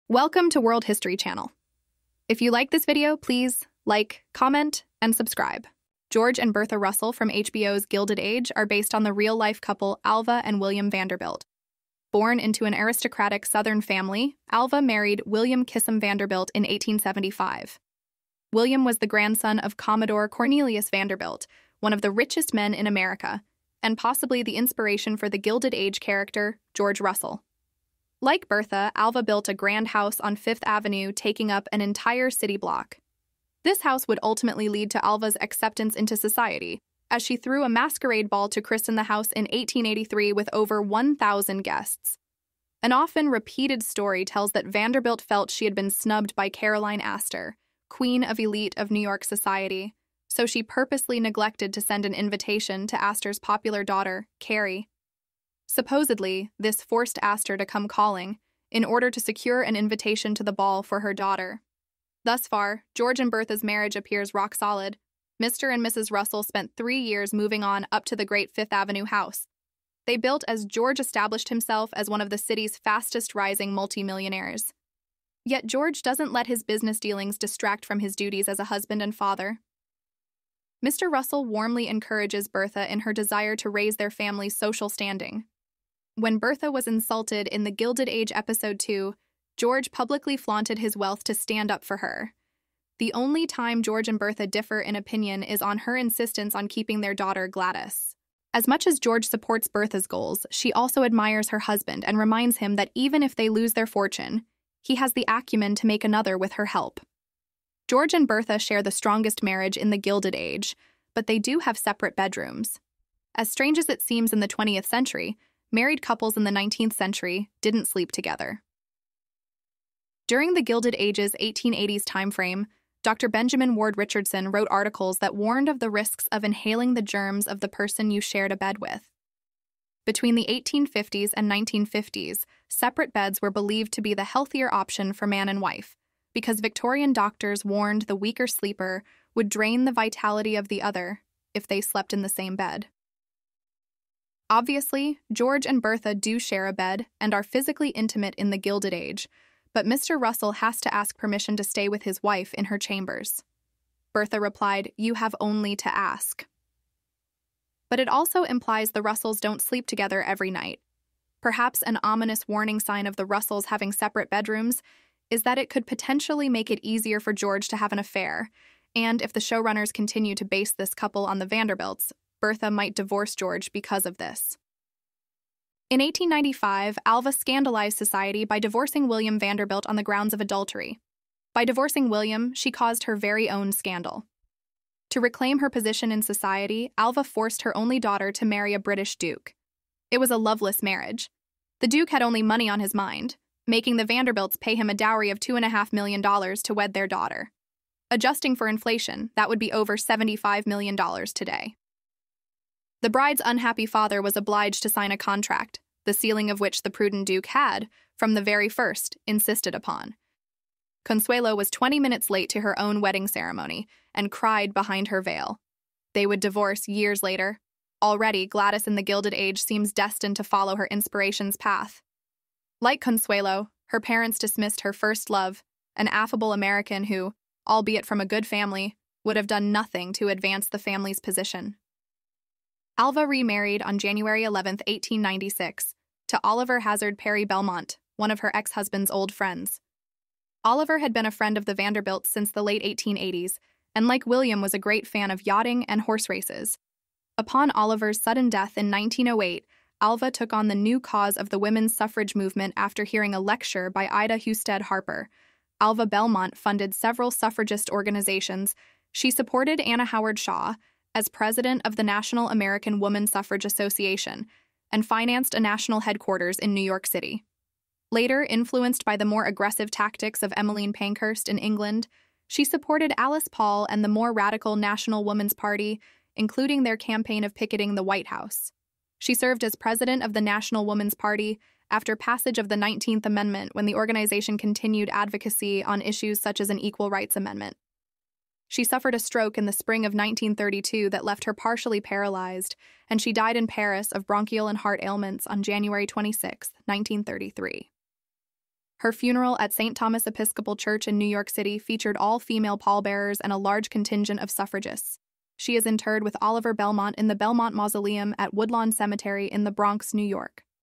Will Bertha and George Russell get divorced in HBO's The Gilded Age season 2
Will Bertha and George Russell get divorced in the gilded age season 2?
The American Gilded Age was a period of immense economic change, of great conflict between the old ways and brand new systems, and of huge fortunes made and lost. Season two of THE GILDED AGE begins on Easter morning 1883, with the news that Bertha Russell's bid for a box at the Academy of Music has been rejected.
Thus far, George and Bertha's marriage appears rock-solid. Mr. and Mrs. Russell spent three years moving on up to the great Fifth Avenue house they built as George established himself as one of the city's fastest-rising multimillionaires. Yet George doesn't let his business dealings distract from his duties as a husband and father. Mr. Russell warmly encourages Bertha in her desire to raise their family's social standing. When Bertha was insulted in The Gilded Age episode 2, George publicly flaunted his wealth to stand up for her. The only time George and Bertha differ in opinion is on her insistence on keeping their daughter Gladys.